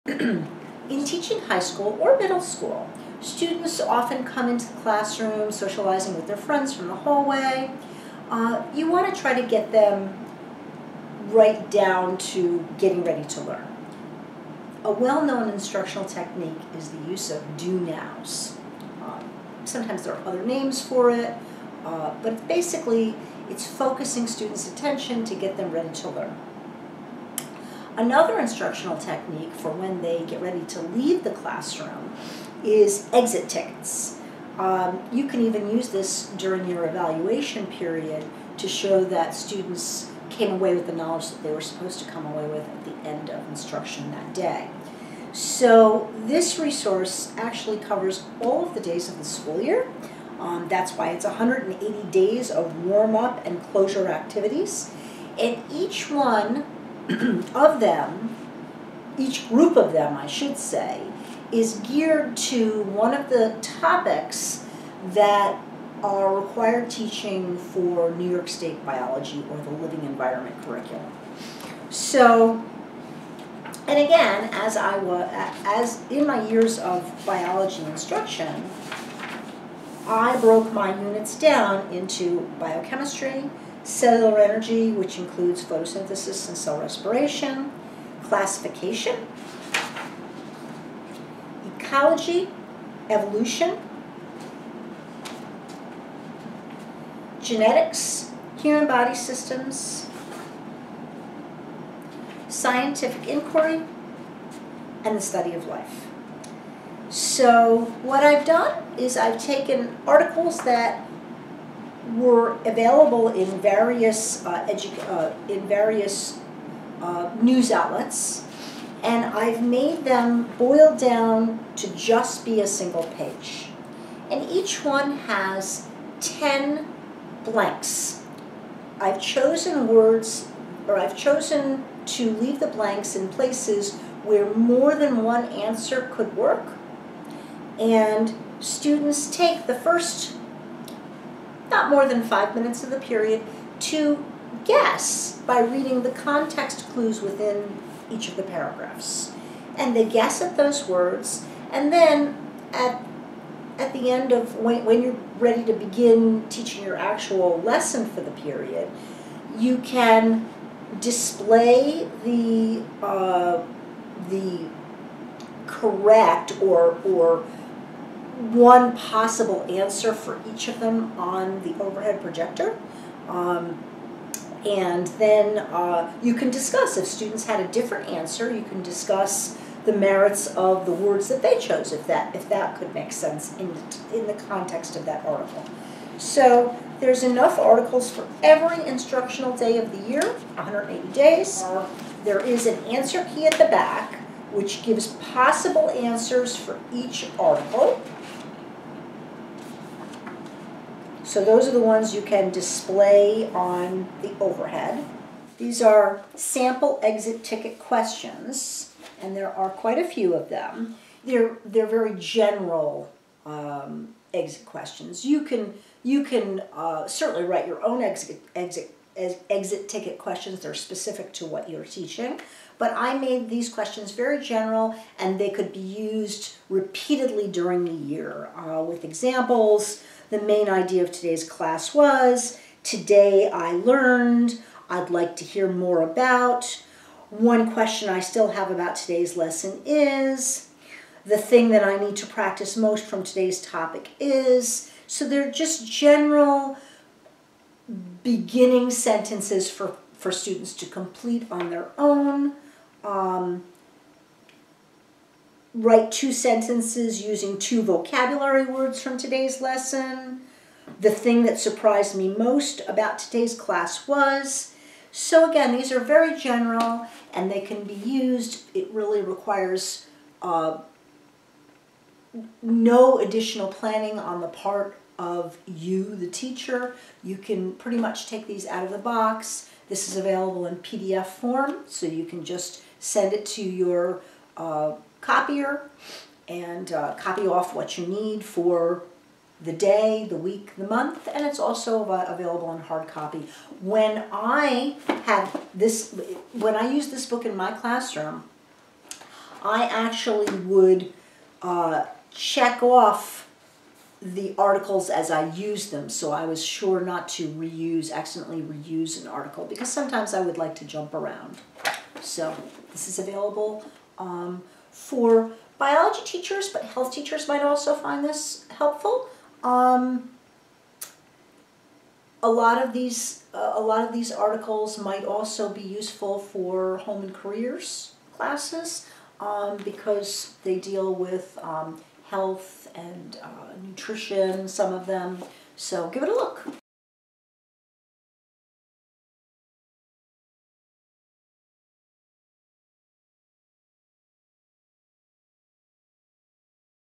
Teach using "Do Nows" and "Exit Tickets"
G. Katz Chronicle talks about "Do Nows" and "Exit Tickets": 180 Days of Warm-Up and Closure Activities.

"Do Nows" and "Exit Tickets": 180 Days of Warm-Up and Closure Activities is a resource that is beneficial for teachers, instructors, and homeschoolers. Copy-paste to find this resource

Purchase STEM books on Amazon